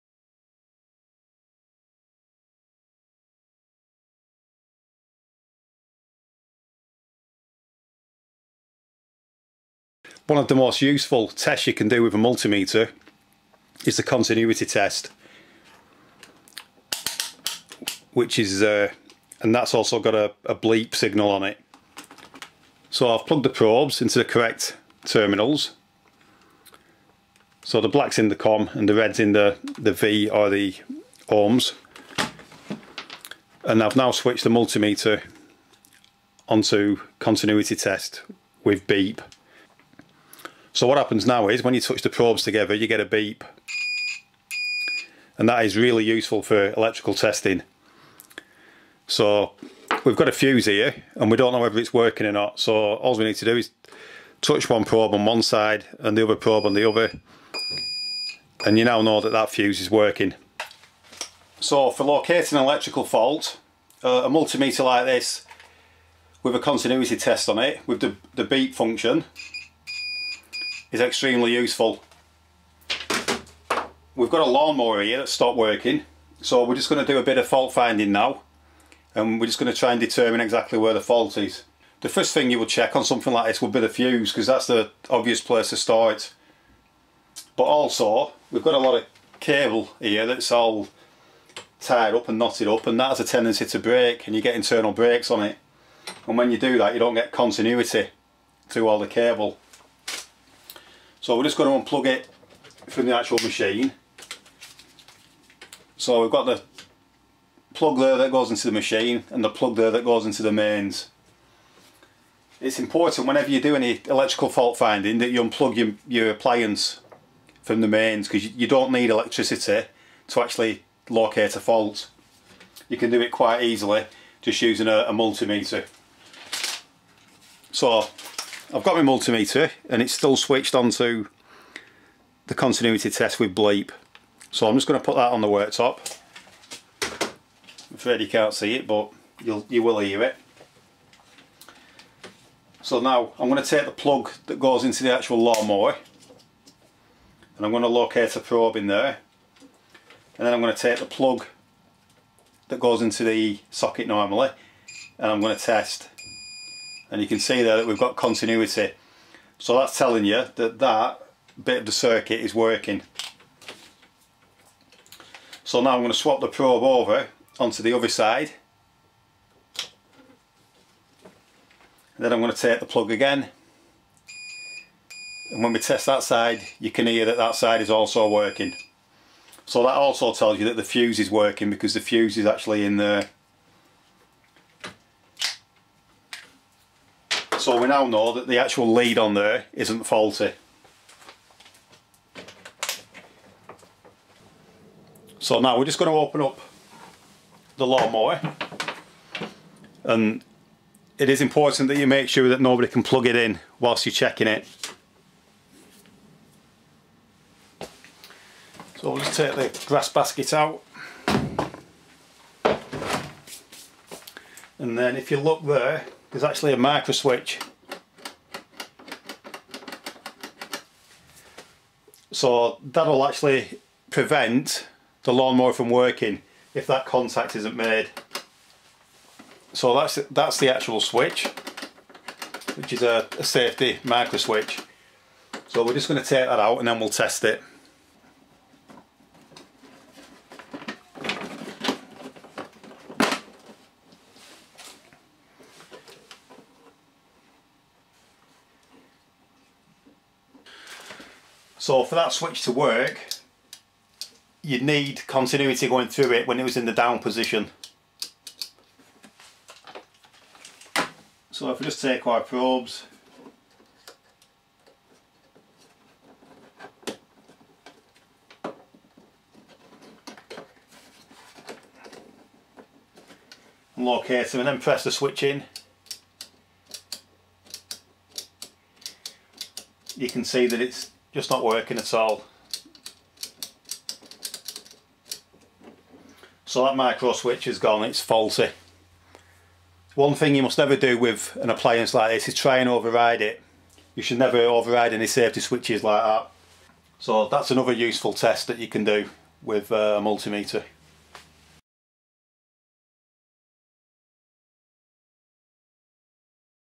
Multi-Meter continuity test
How to use a multi-meter to perform a continuity test This is an extremely useful way to test for faults in electrical circuits.